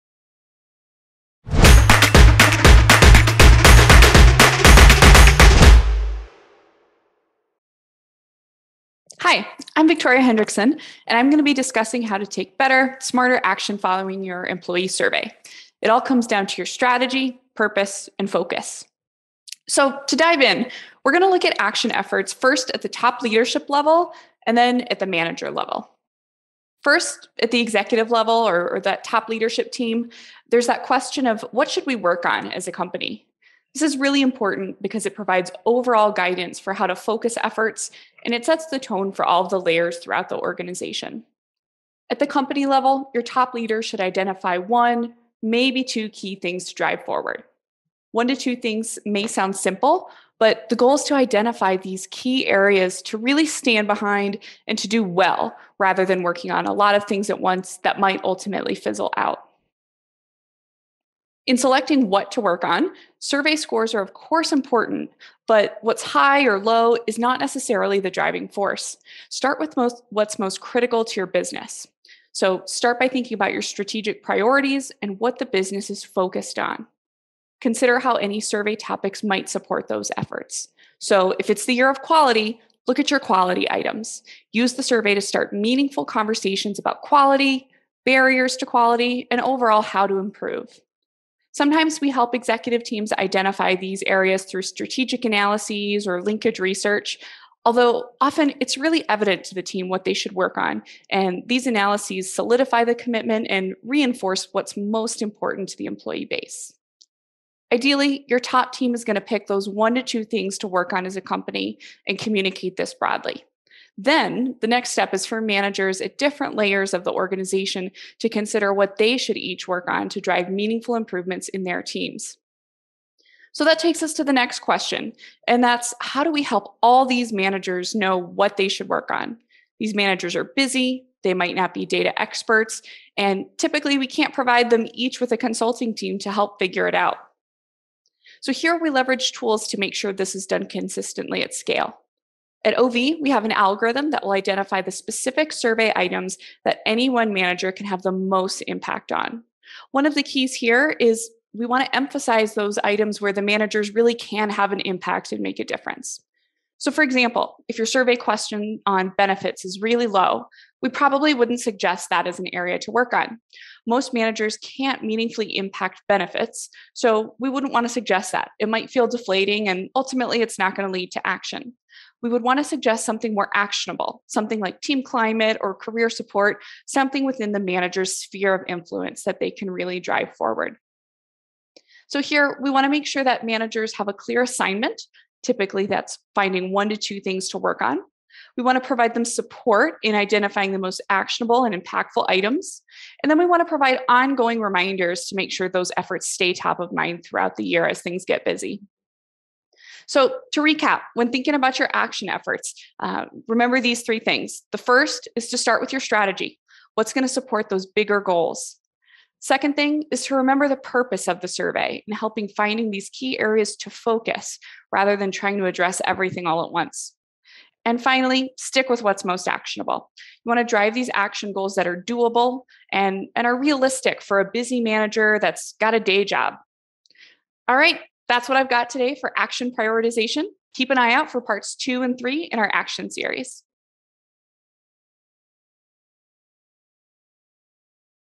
How to Take Action After Your Employee Survey
Employee Surveys are a tool to drive positive change. Designed strategically for your organization, they solicit the data leadership needs to make better decisions and to focus action efforts.

Unfortunately, too often employee surveys are seen as the final destination for organizations soliciting feedback. Yet merely collecting feedback isn’t sufficient; you need to fully understand the data, put it into the context of your unique organization, and create Action Plans around the items that are most likely to have an impact.

This isn’t easy, which is why so many organizations struggle. To make it more attainable, we’re introducing a 3-part web series focused on action. These short videos will cover issues such as where to focus your efforts, how to empower your people to act, maintaining momentum long after a survey closes, and so much more.